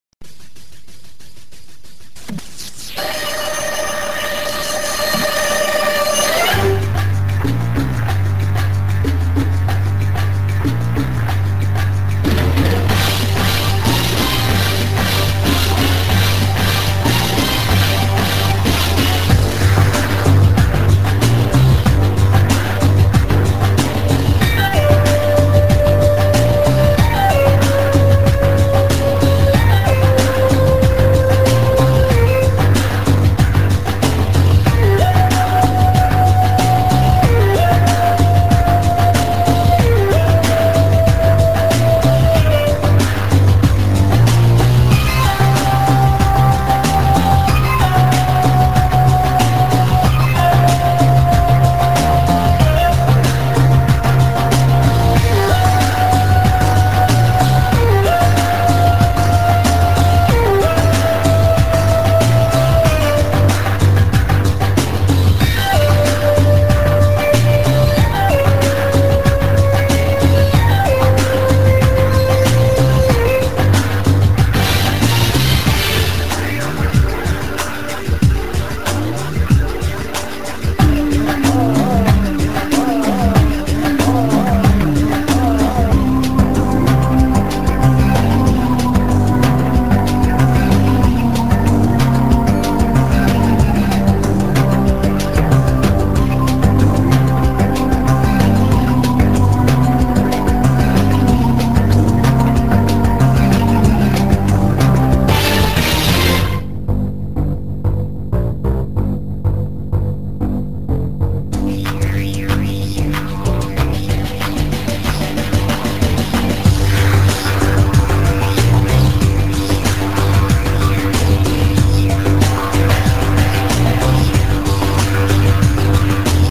Line rider pro!
yet another great liner rider with more cool music a BACKWARDS loop! and more crazy jumps! enjoy!